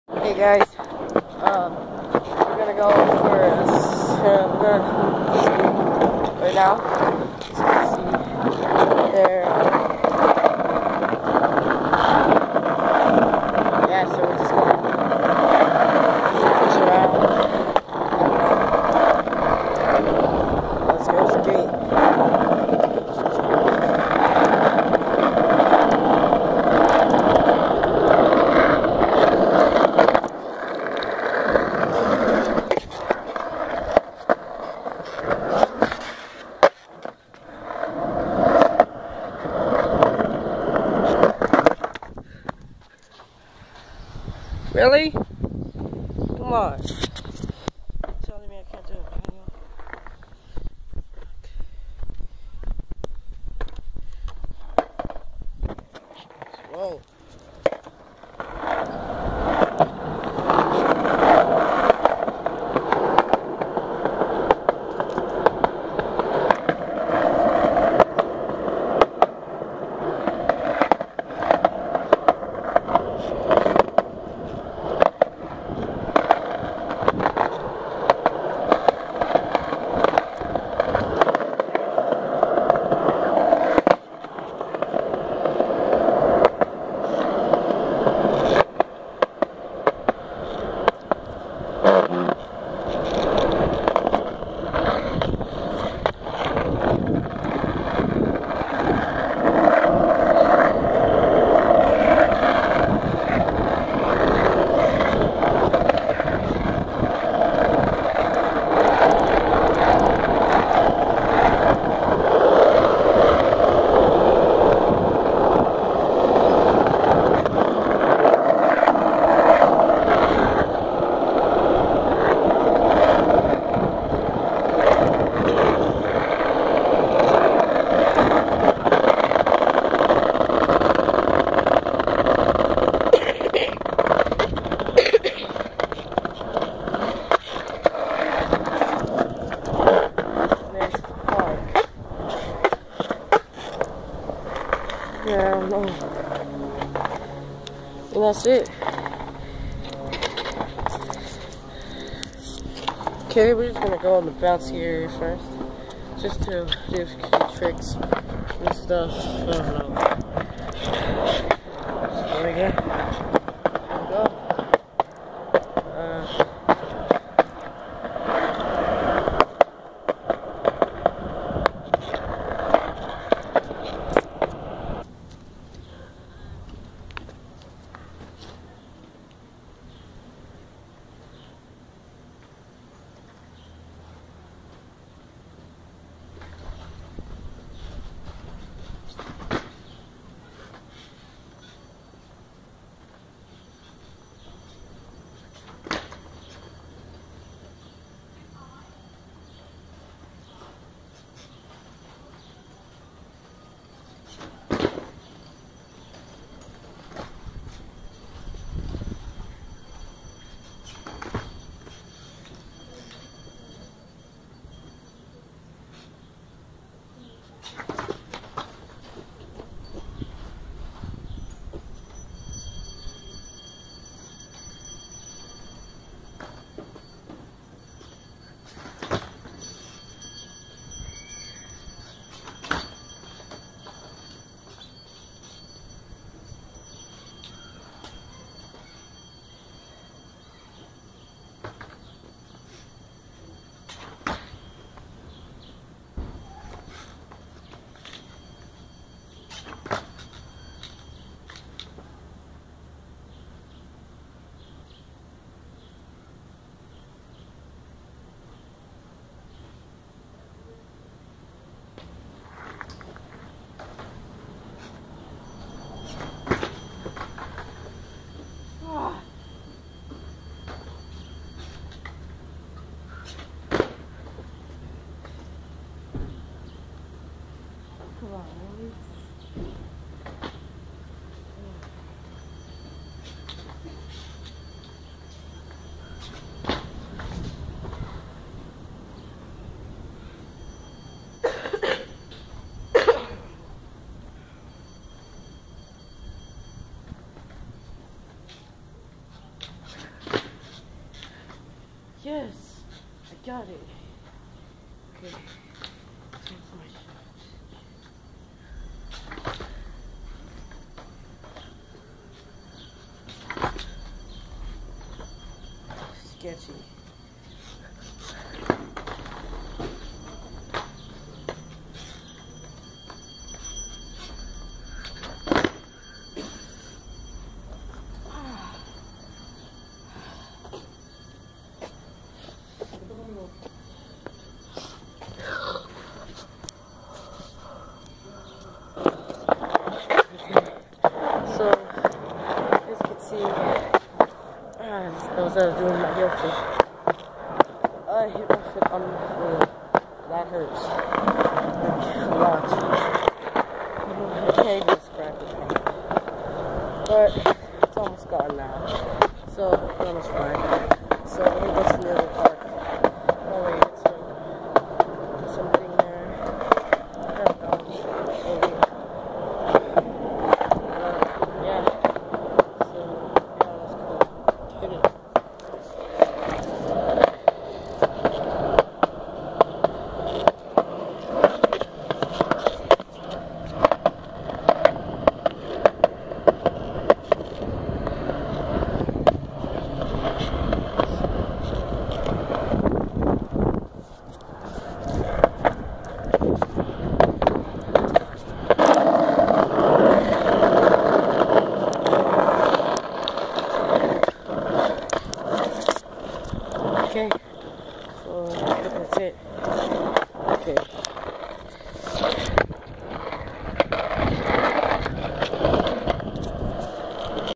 JUST SKATING
This is a video of me riding my skateboard in my neighborhood. Part two is in a seperate video.